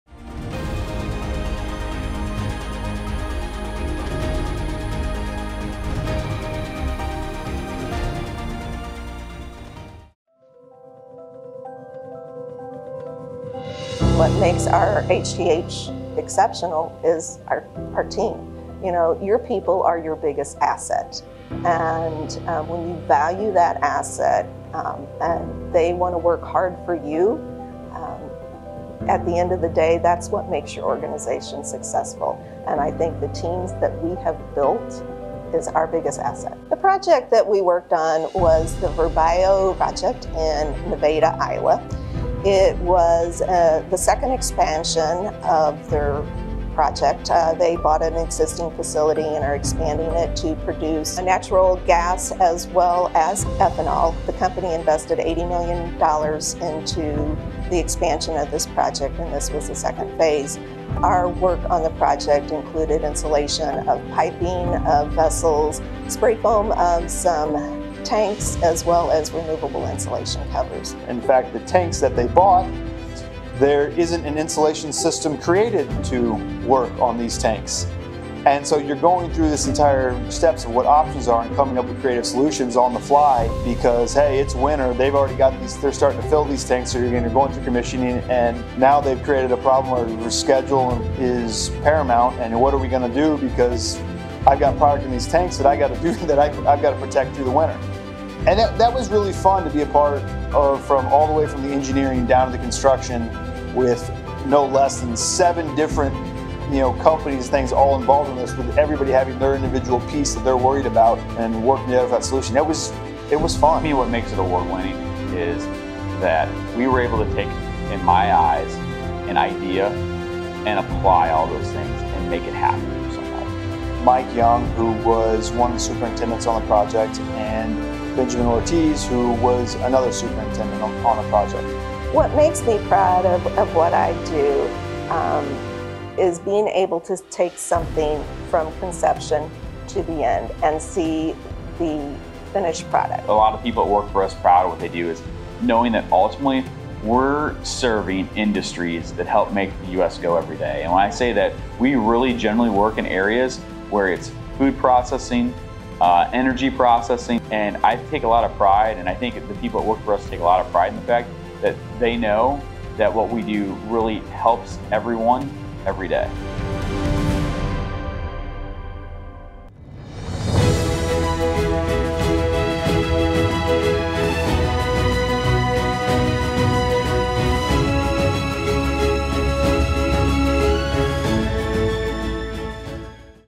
2024 EIC Awards | TOP SUBCONTRACTOR | hth companies, inc. | Verbio NBR Insulation
SUBCONTRACTOR PROJECT OF THE YEAR
2024 EIC Subcontractor Category Winner: Mechanical Industrial (Under $2 Million)